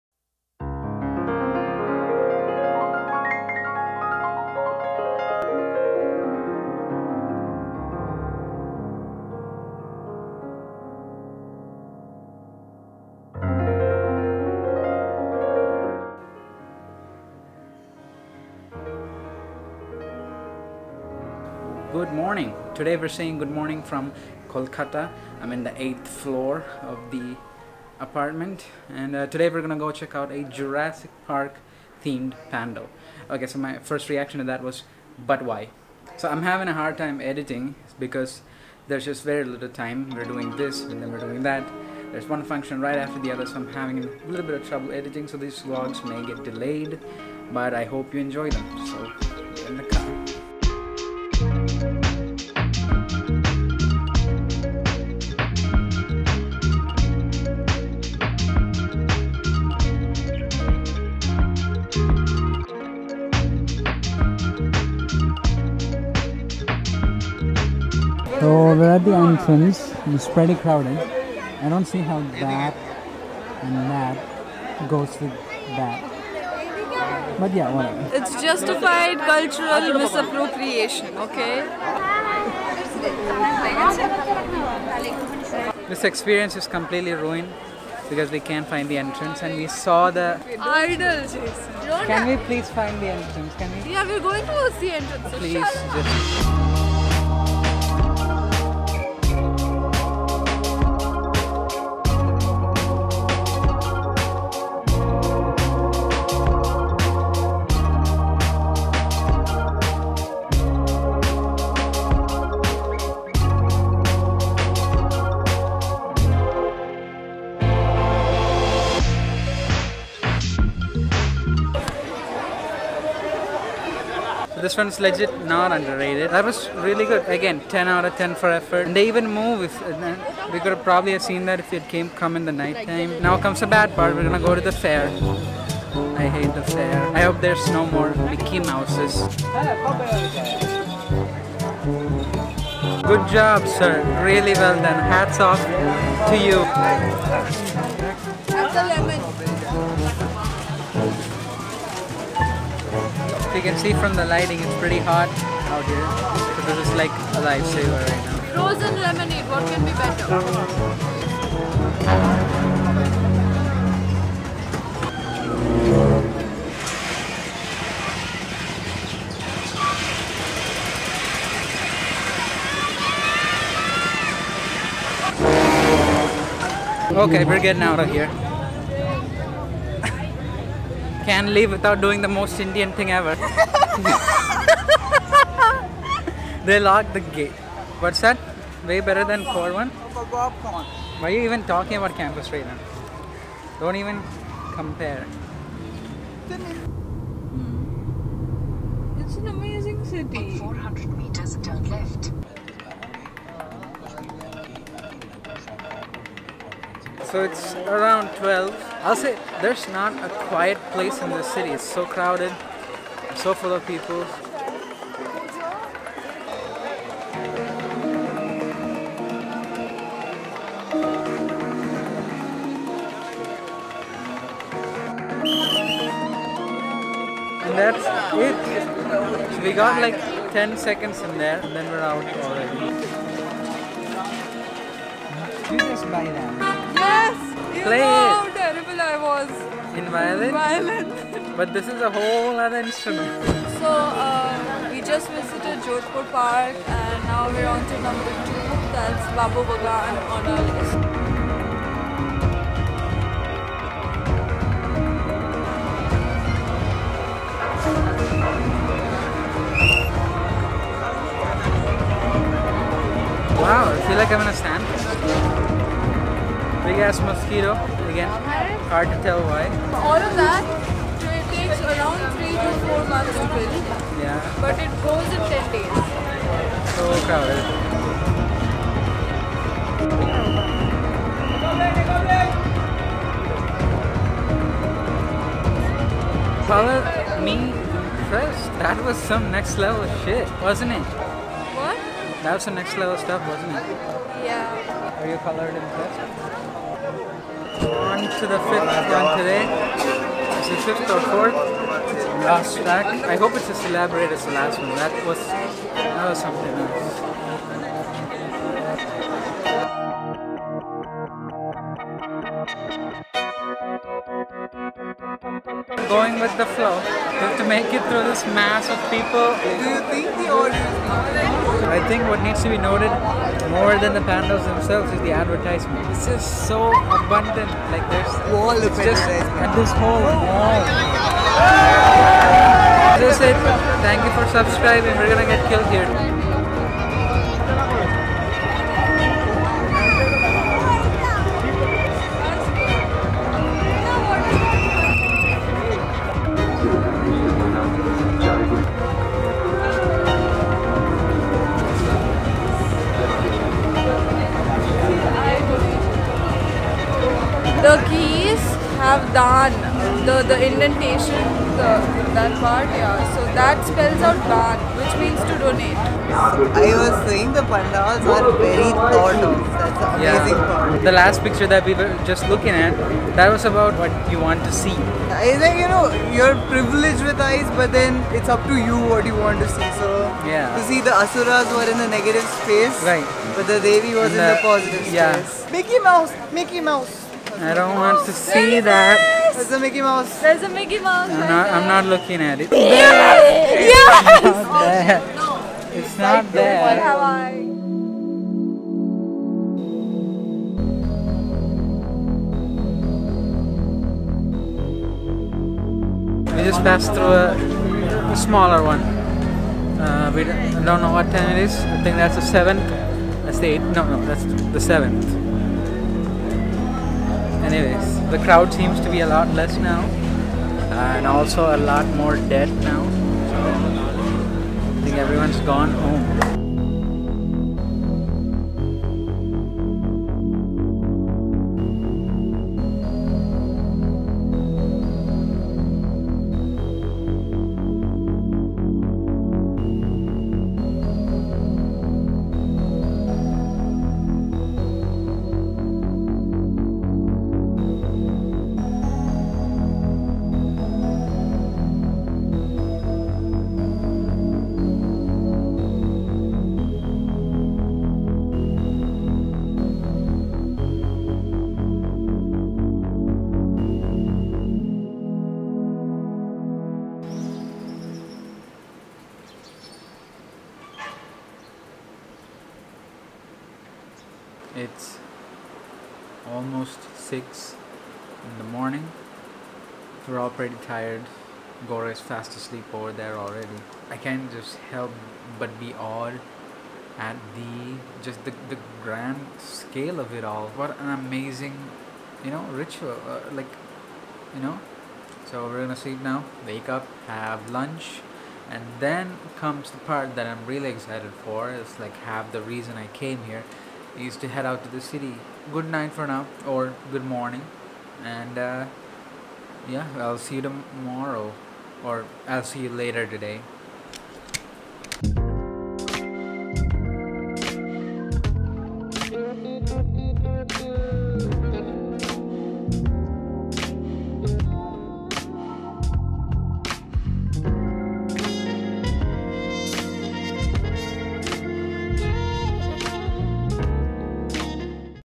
YOU’LL NEVER BELIEVE HOW HUGE THIS IS | Day 2, Kolkata
Hey! This is the complete experience of my first visit to Kolkata. I had a lot of fun in the City of Joy during Durgapuja this year! Went pandal hopping, visited the Victoria Memorial and had lots of authentic Bengali food.